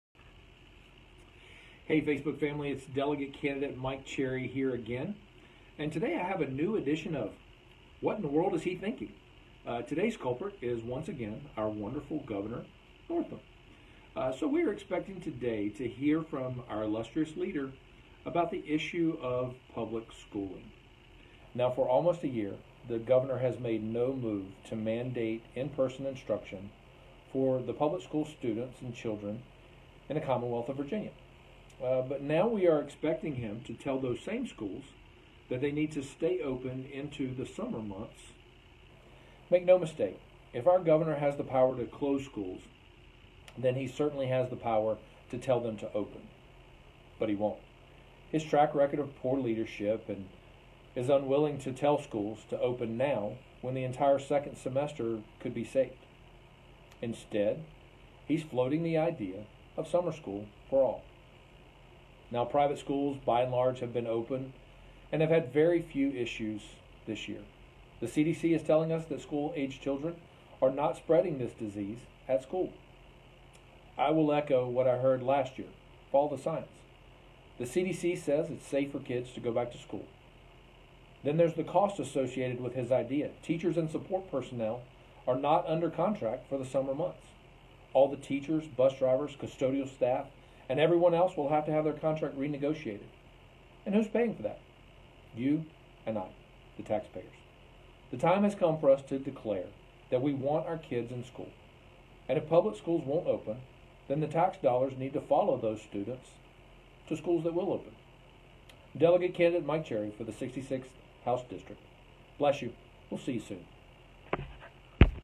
Governor Northam summer school idea
Well, ladies and gentlemen, we are expecting today for Governor Northam to recommend today that public schools stay open into the summer months. The same governor that will not mandate schools open for face-to-face instruction now wants the same schools to stay open longer. Makes no sense...